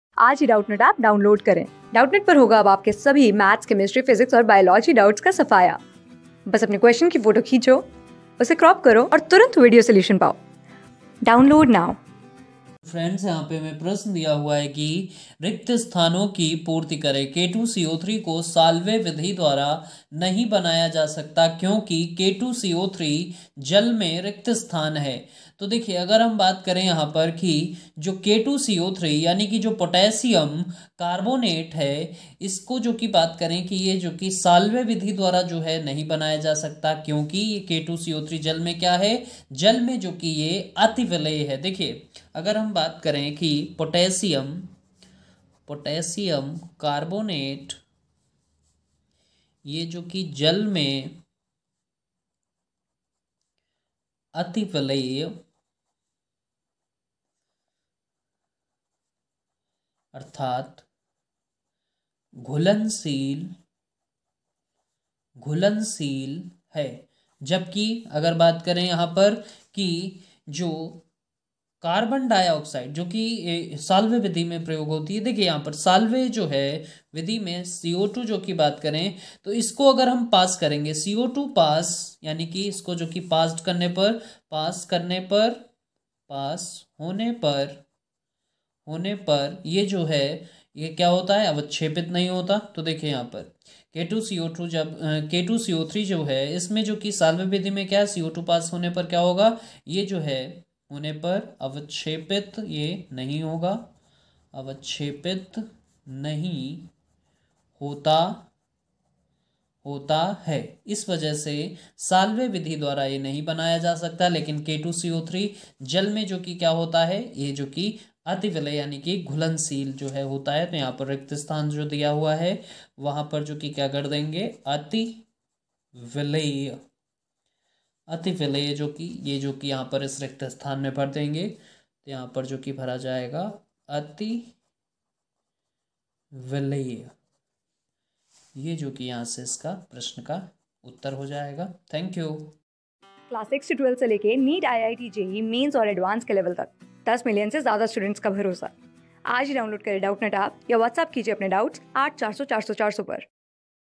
रिक्त स्थानों की पूर्ति करें K_(2)CO_(3) को सॉल्वे विधि द्वारा नहीं बनाया जा सकता क्योंकि K_(2)...
रिक्त स्थानों की पूर्ति करें K_(2)CO_(3) को सॉल्वे विधि द्वारा नहीं बनाया जा सकता क्योंकि K_(2)CO_(3) जल में है
Class: 11
Subject: CHEMISTRY
Chapter: s-ब्लॉक तत्त्व
Board:Bihar Board

👉 Have Any Query? Ask Us.